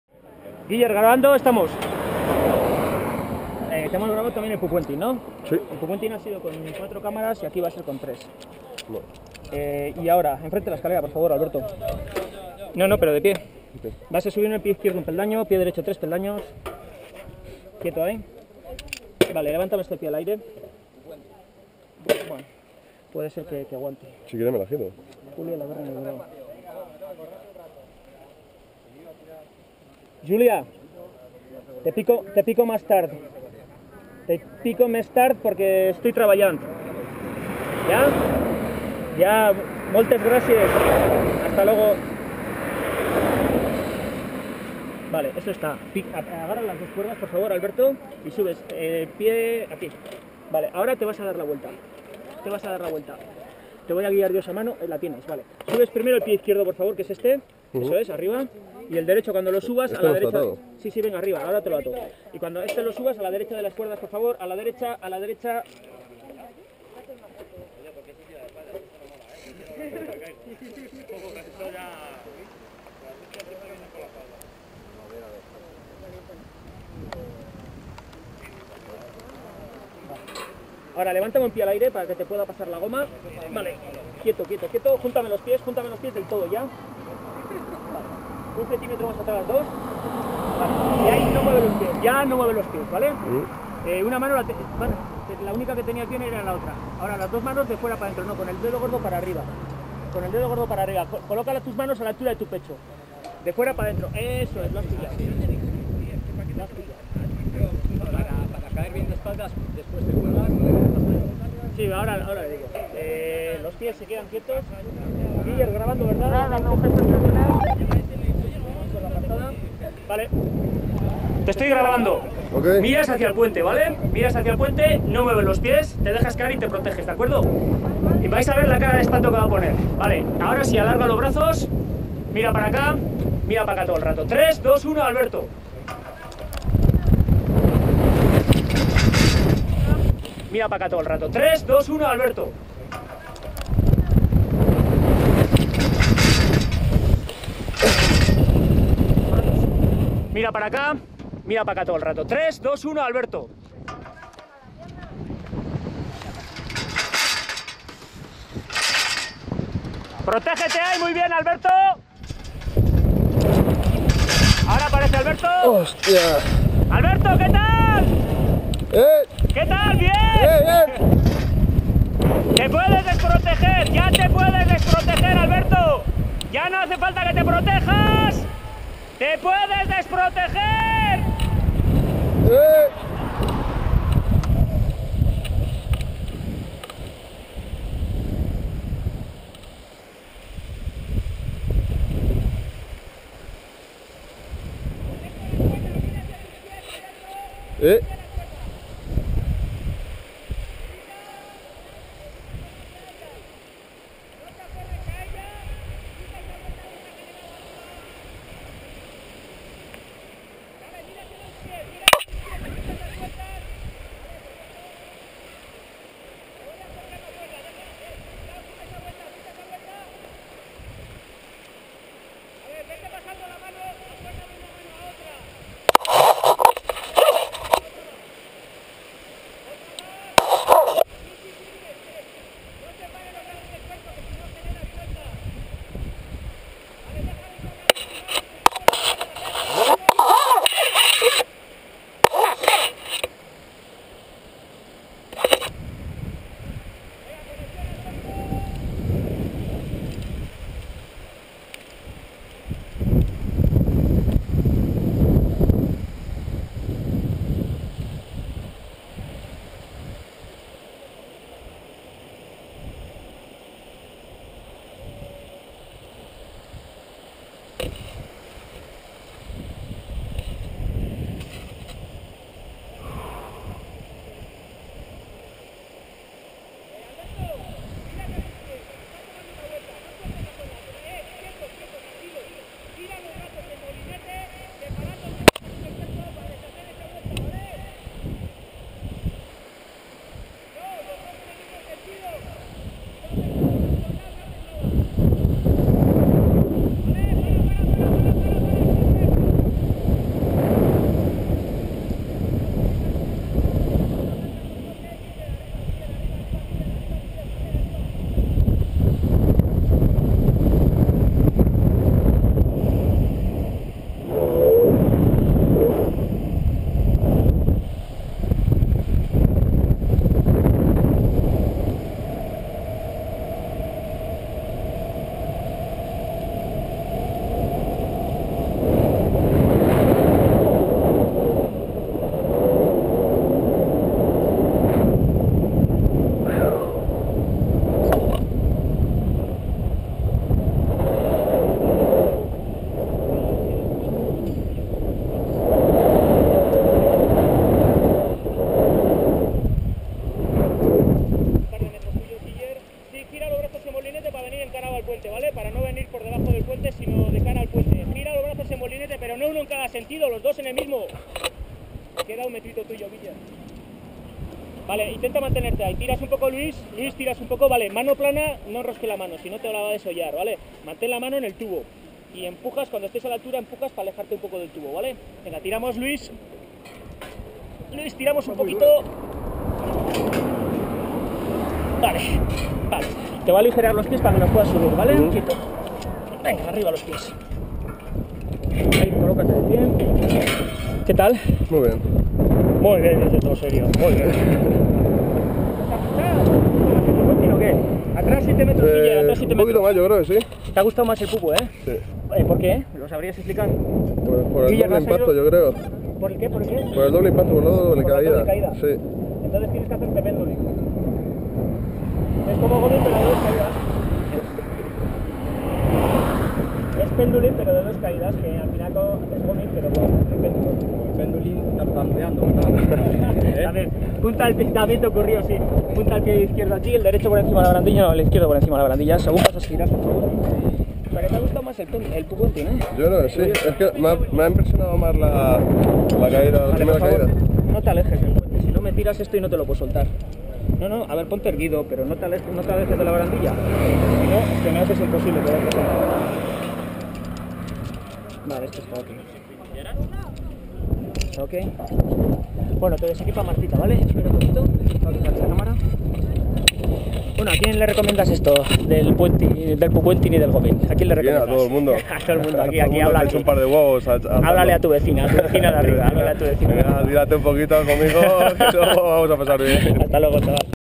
Góming, Bungee Jumping en Cofio, Madrid 3c AHG de espaldas dejándose caer
El góming es rock & roll mientras que el puénting es vals. El góming (salto con elástico) es la experiencia más adrenalínica que una persona común y sin preparación especial puede hacer, siempre que cumpla unos mínimos de estado de salud y de forma. Prácticamente cualquiera puede hacer góming y el de Cofio es uno de los más altos y largos que se pueden hacer en España. Los hay (y los tenemos) más grandes, pero son una o unas pocas veces al año.

En este caso, nuestro cliente ya era experimentado, había hecho góming lanzándose de cara (la que consideramos postura básica) y quiso probarlo dejándose caer de espaldas. En este salto recomendamos encarecidamente que la persona mire al puente mientras cae, por una parte para que vea cómo se aleja el mismo, un efecto muy impresionante, lo de verlo alejarse, así, a la vez, no ve dónde cae, lo que combinado con la ingravidez de los primeros 18 metros, hace que la sensación sea muy muy fuerte. Además, haciendo eso (lo de mirar al puente mientras cae) podemos ver y grabar la cara de pánico que pone la persona en ese momento de no ver hacia dónde cae y no sentir nada que la sujete.

Muy heavy.

¿Te animas?